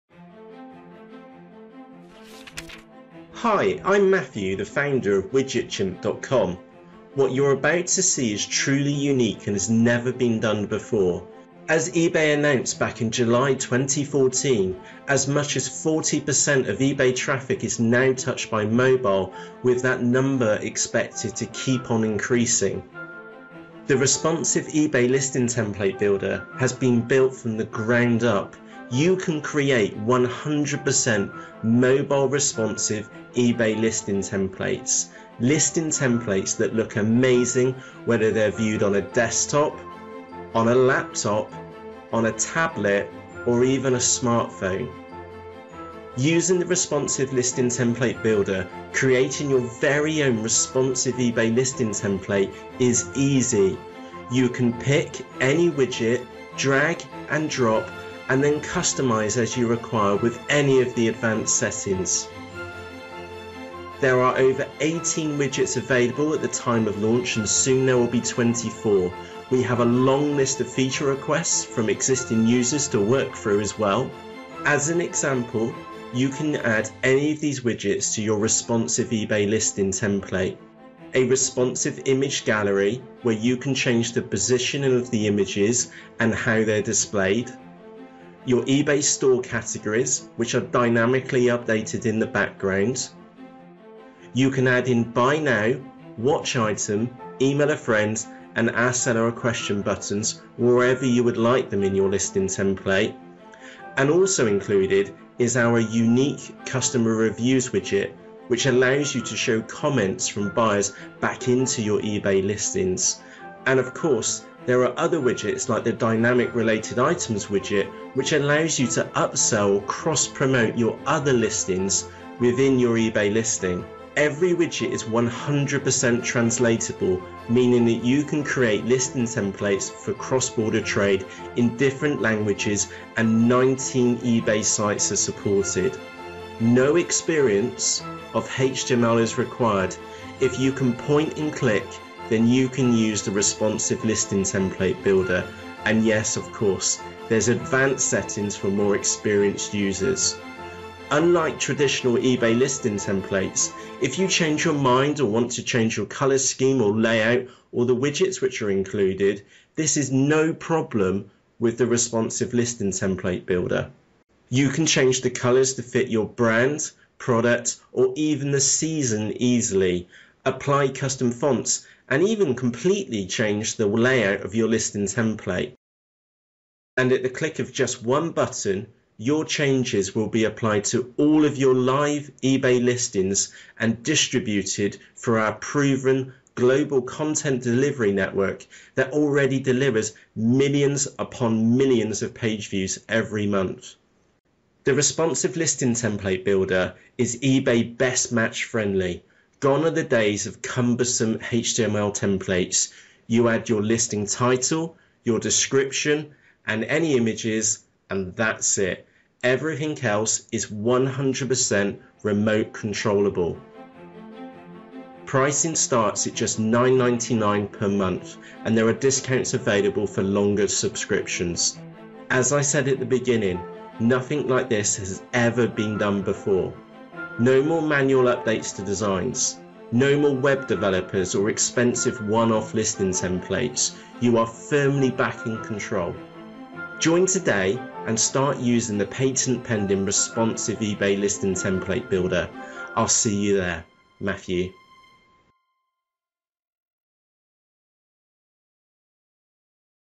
eBay Responsive Listing Template Builder - WidgetChimp
Over 40% of eBay sales are now touched by mobile devices according to eBay with over 305 million downloads of their mobile apps.

With the Responsive template builder you can create an eBay listing template that works on all devices including iPhones & iPads.

Then remote control the layout, widgets & colour scheme remotely.

- No more expensive designer fees
- No more geeky HTML
- Change your template at any time & update listings instantly

Remote Control Your eBay Listings

Once listed on eBay, you can "remote control" the layout, colour scheme and widgets in your listing template, make your changes, press one button and all of your eBay listings that are using the template are updated.

No more manual revisions or waiting for 3rd party software to update. One click and the entire template is updated instantly.

For example for Christmas this could be changing your template to a green colour scheme, then in January a red theme for a sale, then pink for Valentine's day and back to any colour scheme you desire for the rest of the year.

Each time updating them remotely by clicking one button to update all your live listings using the template(s).

Unique Widgets Only Available in WidgetChimp

There are 27 "Drag & Drop" widgets available for your eBay listing templates, here are just a few of them:

- Responsive Image Gallery
- Dynamic Related Products
- eBay Item Specifics
- Customer Reviews
- Buy / Watch / Email Buttons
- eBay Search Box
- Image / Banner Slider
- Product Videos
- Seller Report Card
- Feedback Slider
- There are +15 other widgets available

All 3rd Party Listing Tools Supported

M2E Pro, SixBit, Linnworks, ChannelAdvisor, eSellerPro, BrightPearl, SellerExpress, ChannelGrabber, ChannelUnity, eBay Selling, Manager Pro, eBay Turbo Lister, StoreFeeder, OpenBay Pro, Auctiva, Exact Abacus, InkFrog & InkFrog Open & Neto

If you eBay listing tool is not mentioned here, just contact support for more details. Adding additional eBay listing tools is completed within 1 business day.

Perfect for Cross Border Trade

Each of the widgets have been professional translated into English US, English UK, French, German, Spanish & Italian. Changing the language is easy, it's a dropdown box.

During the "Black Friday" weekend in 2015, we received 35,786,702 hits with zero downtime.

WidgetChimp leverages a global Content Delivery Network which means that your listing template will load fast no matter where your customers are.

WidgetChimp is directly integrated with eBay. Joining takes just 4 clicks.

To find out more about the Responsive eBay listing template builder and to join right now in just 4 clicks, start